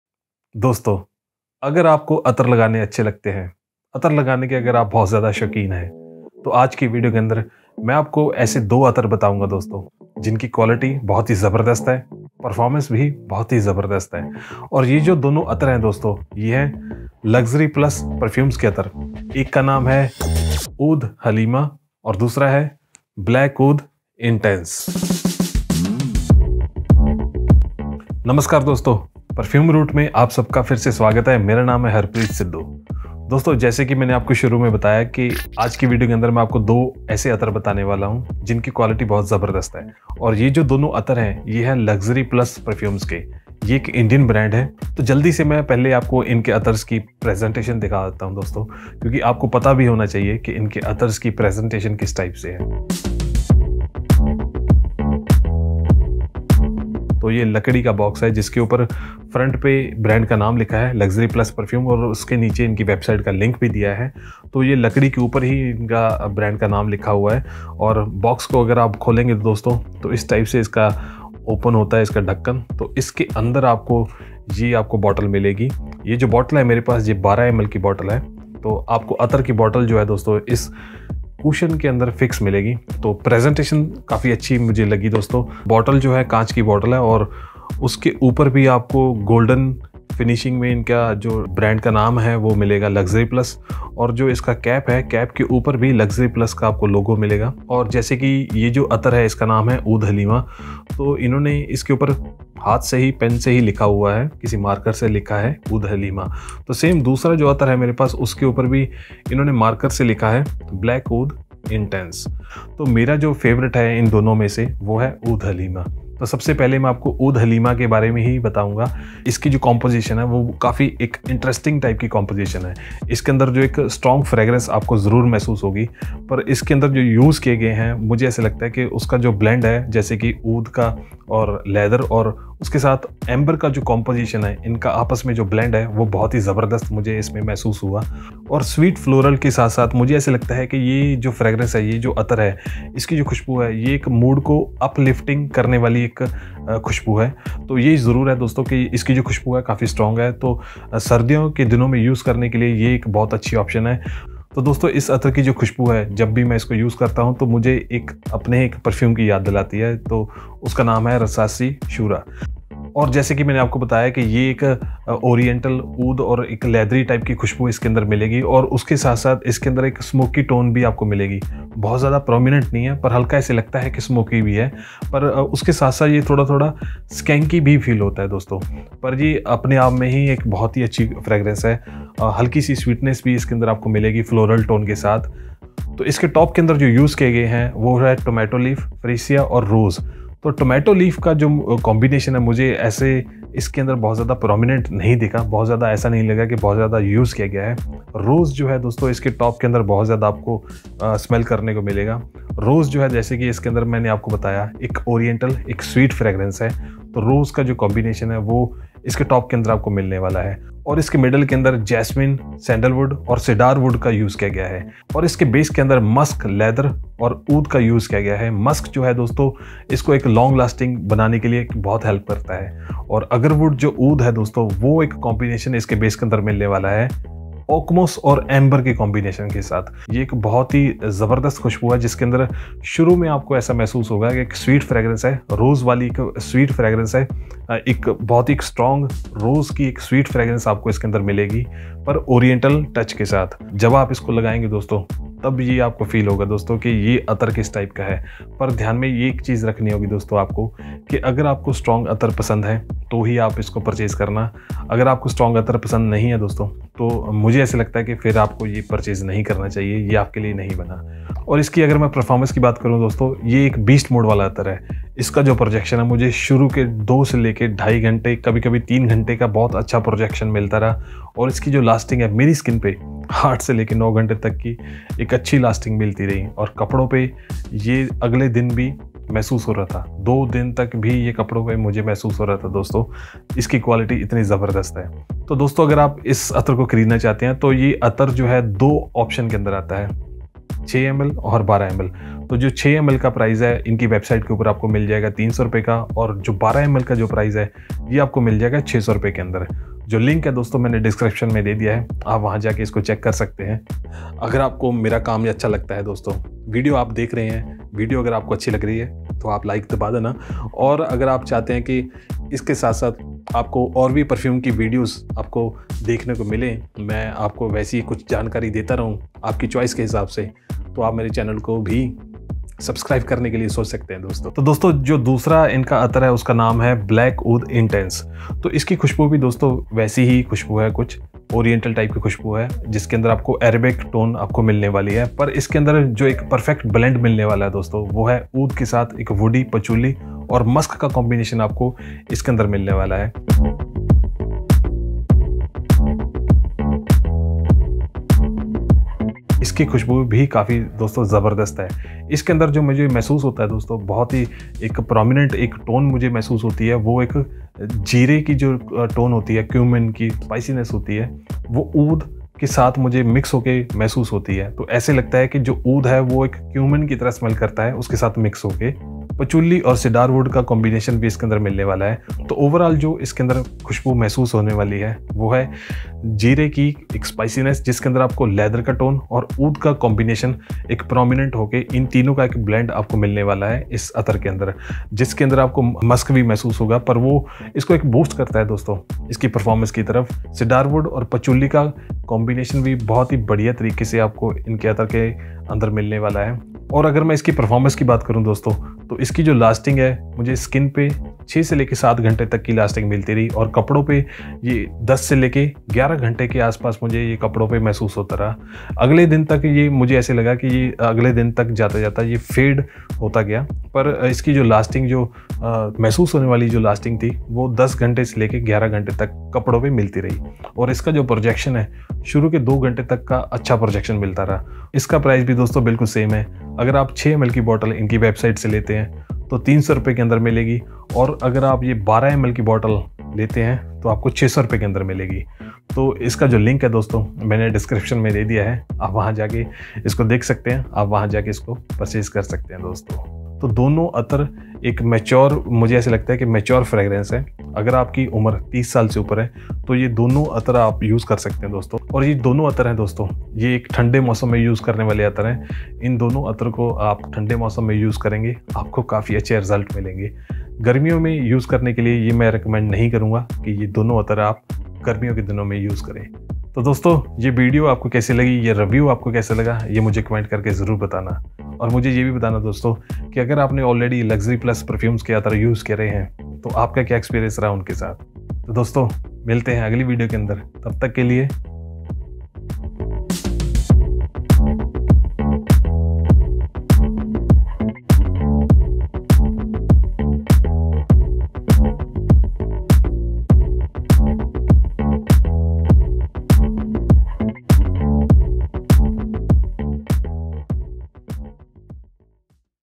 Long lasting attar by Luxury Plus Perfume 🎉👌Oud Halima & Black Oud Intense Attar Review
🎉🎉Oud Halima🎉🎉
Oud Halima by Luxury Plus is a Sweet Oriental Oudi Leathery Arabic Fragrance for both Men and Women.
Top Notes Are Tomato Leaf, Freesia, Rose.
Middle Notes are Jasmin, Cedar Wood, Sandal Wood,
Base Notes Are Musk, Leather, Agarwood (Oud) Oakmoss and Amber.

It is a perfect choice for Strong Arabic Attar lover for personal use or gifting.
A Pure High Quality Non-Alcoholic Undiluted very long-lasting Attar.
🎉🎉Black Oud Intense🎉🎉
Black Oud Intense by Luxury Plus is an Oriental Strong Arabic Fragrance. A perfect blend of Woody oil mixed with Patchouli and Musk
It is a Pure High Quality Non-Alcoholic Very Strong Attar.
,,,,,,,,,,,,,,,Available Size,,,,,,,,,,,,,,,
12 ML in Bottle with Box
6 ML in Bottle with Velvet Pouch.

00:00 Jee Aayan Nu
00:27 Intro
01:59 Oud Halima
05:25 Black Oud Intense
🔥🔥Reviews🔥🔥
🔥Arabian Aroma Perfume Review | Best Budget Perfume for Men in India 2023 | Top 5 Perfume for Men